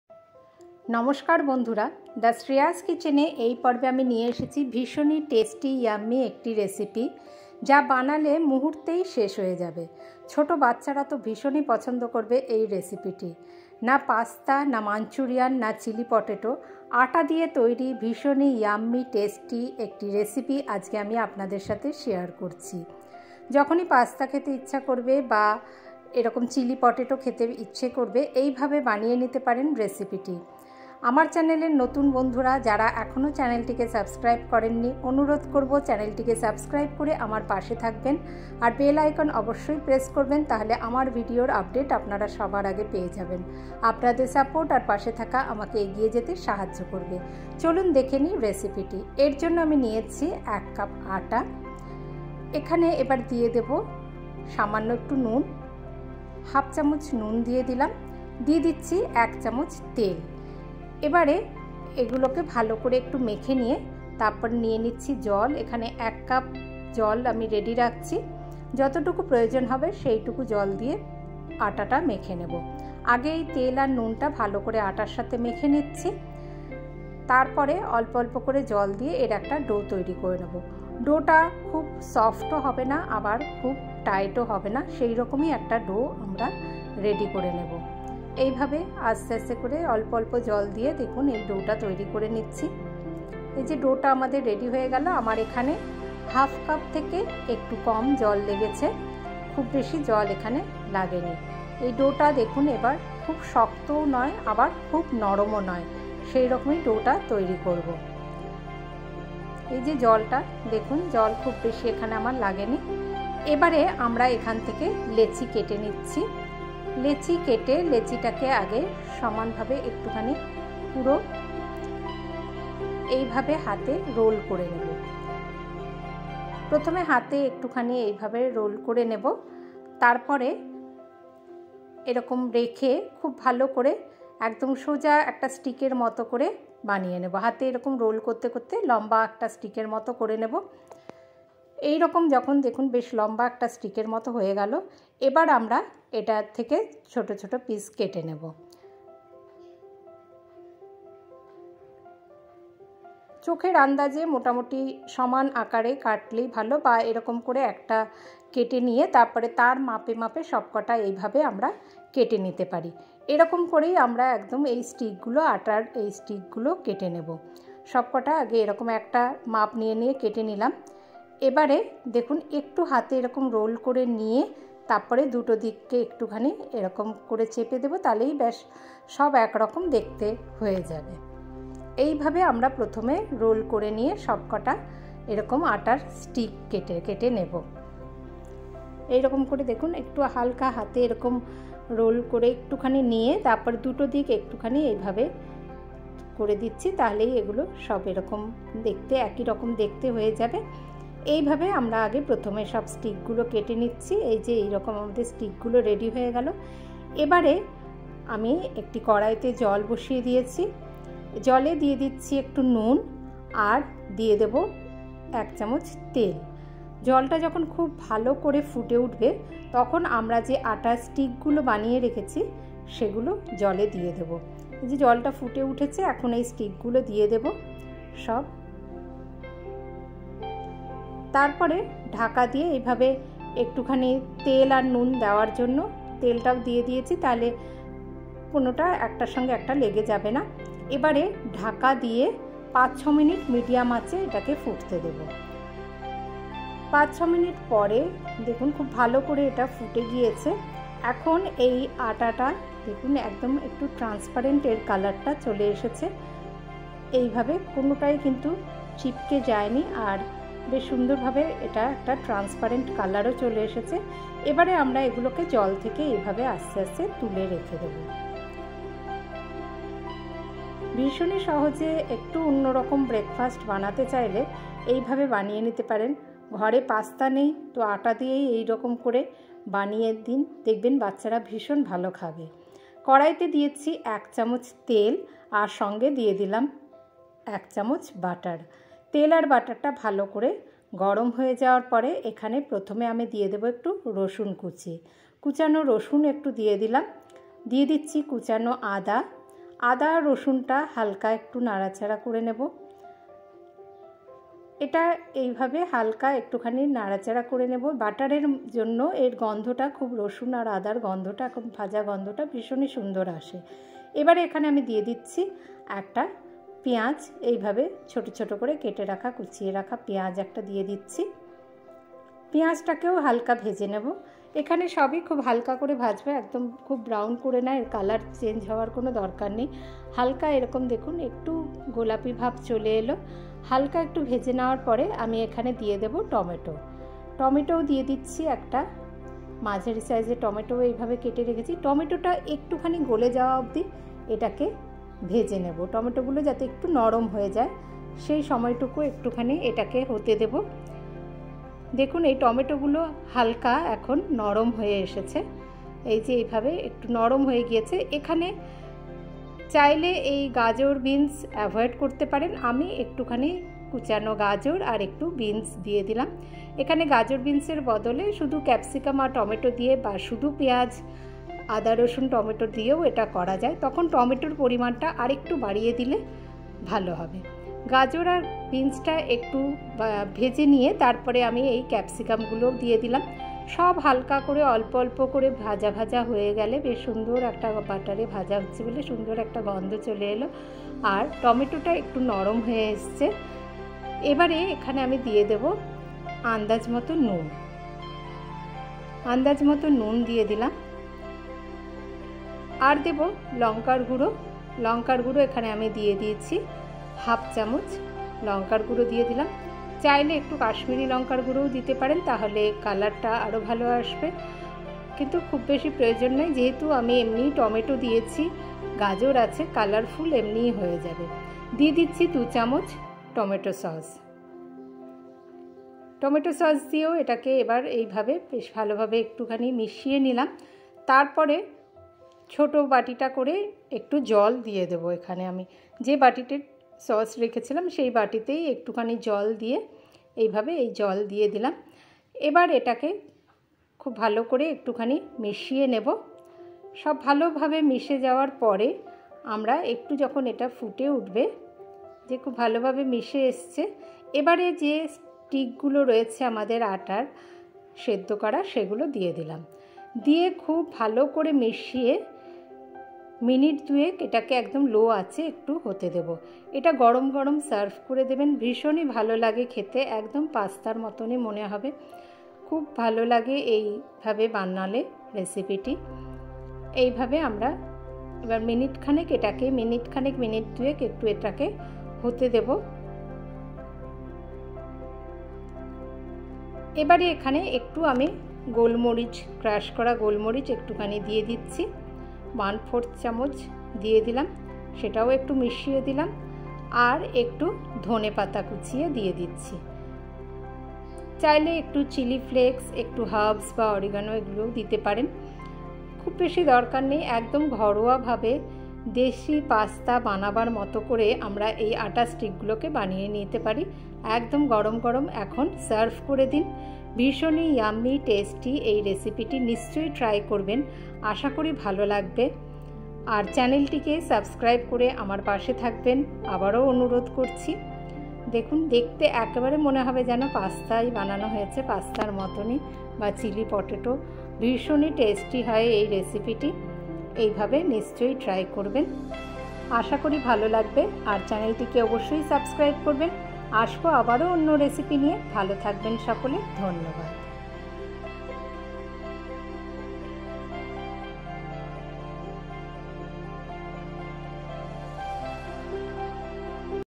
1কাপ আটা দিয়ে তৈরি অজানা Yummy রেসিপি বানালে মুহূর্তেই শেষ হয়ে যাবে।No Pasta, Wheat Flour Recipe
আটা দিয়ে তৈরি অসাধারণ স্বাদের ইউনিক নাস্তা রেসিপি যা সকলের অজানা।
সম্পূর্ণ আলাদা স্বাদের ইউনিক রেসিপি।
আটা রেসিপি।